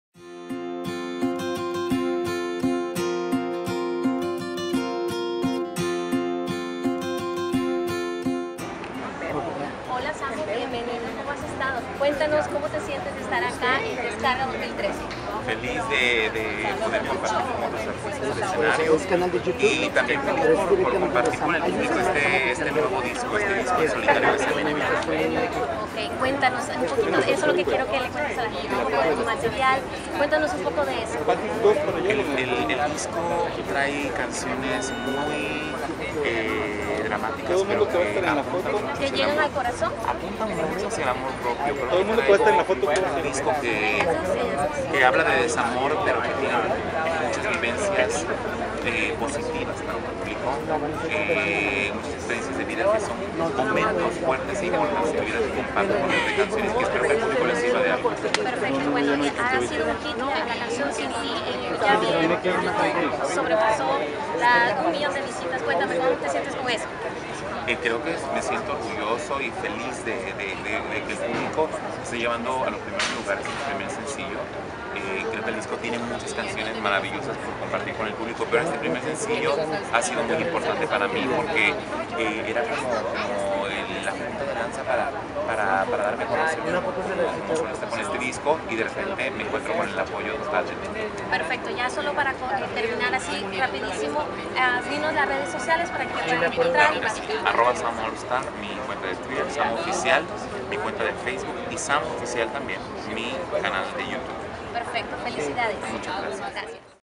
SAMO y su música
SAMO estuvo en la Descarga 2013 en CityWalk de Los Angeles presentando su nuevo material nos habla de su música.